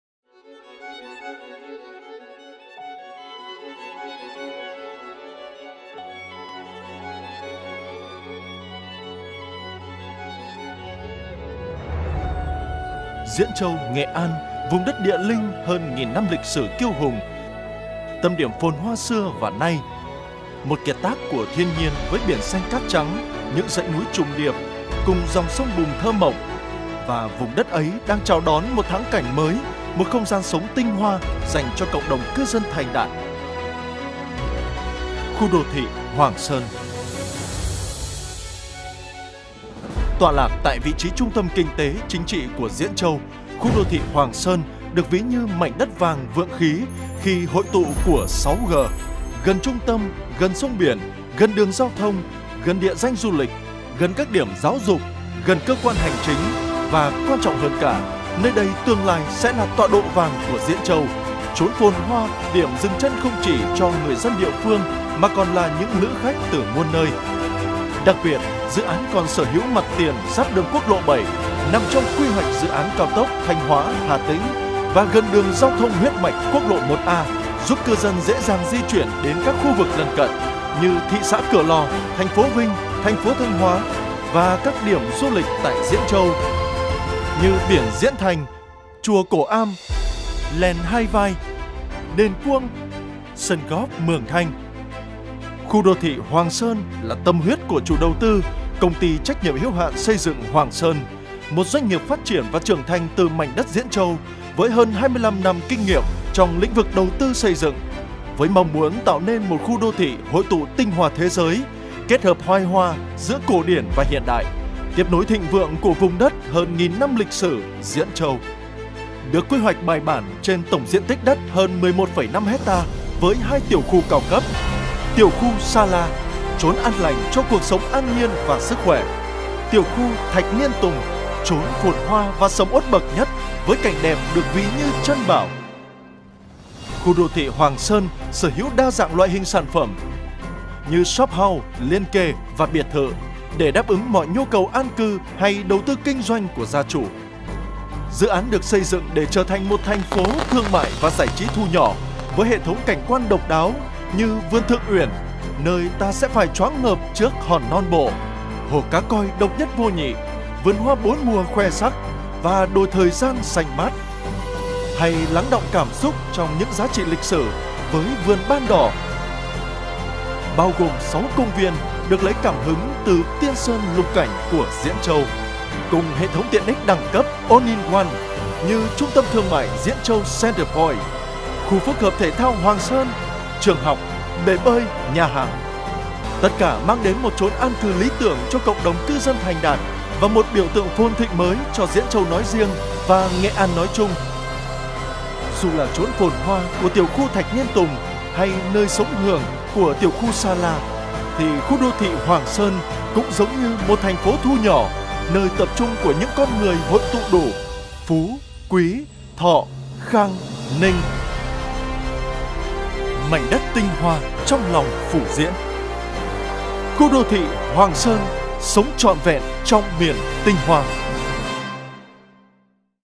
Video giới thiệu dự án Khu đô thị Hoàng Sơn Nghệ An - CAFELAND.VN
Khu đô thị Hoàng Sơn Nghệ An do Công ty TNHH Xây dựng Hoàng Sơn đầu tư trên khu đất rộng 115.231,9 m2, cung cấp ra thị trường 409 lô đất có diện tích từ 90 - 517 m2.